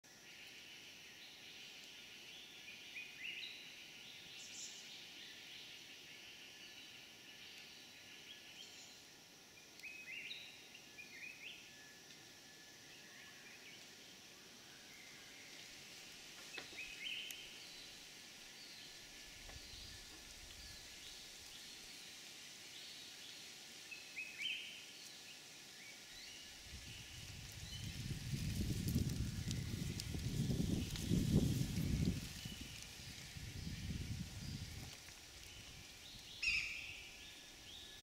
Besra (Accipiter virgatus), Mondulkiri, Cambodia. 20160306_081114.m2ts
Besra (Accipiter virgatus) at a small waterhole in a dry stream bed during the dry season, Phnom Prich Wildlife Sanctuary, Mondulkiri, Cambodia. IUCN=Least Concern.